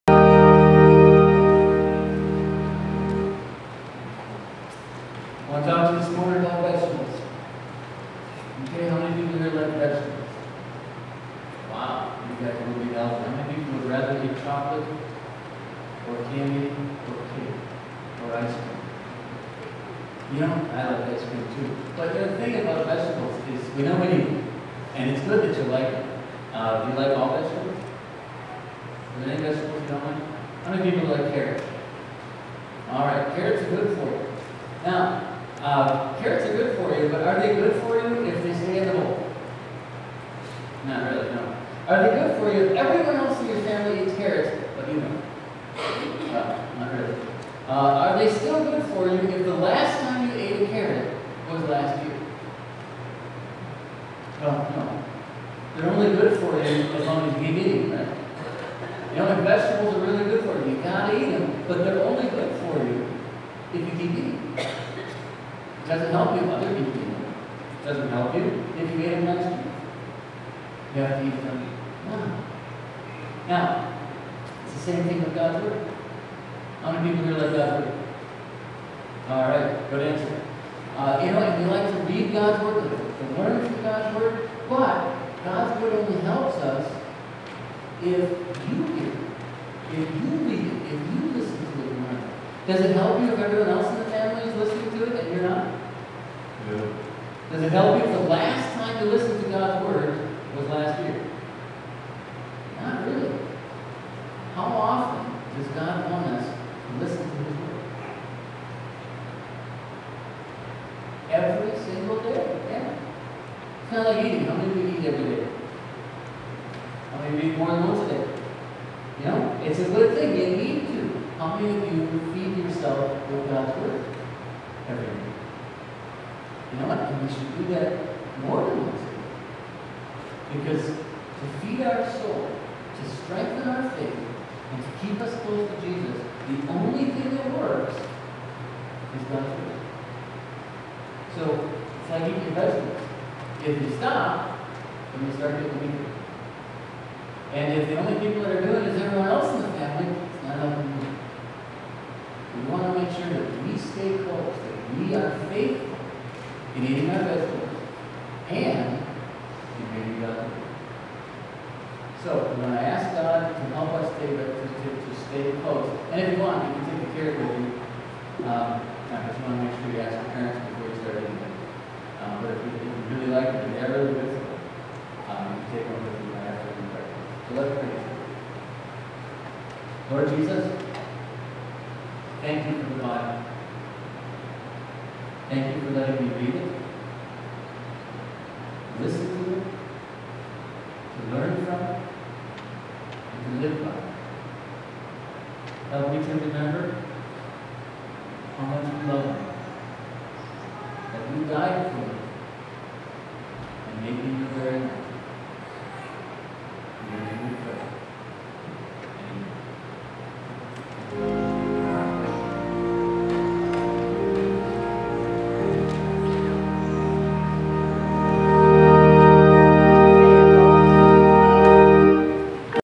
2012 07 15 Children's Sermon Eat Your Vegetables
Vegetables are good for you. But, they don't help if you have them in the house but you don't eat them. They don't help you if everybody else eats them but you don't. They don't help if you used to eat them but aren't eating them now. It's the same with God's Word. The Bible is life, but if it's in your house and you're not reading it, it won't help you. It doesn't help if everybody else is reading and you're not. It doesn't help if you used to read it but aren't reading it any more. Read the Bible. Eat your vegetables. Daily.